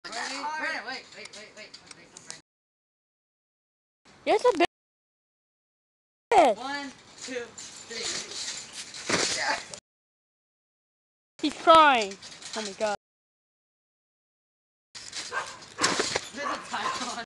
choke slam through a (ghetto)table
me and florentino chockslamming pedro though a piece of wood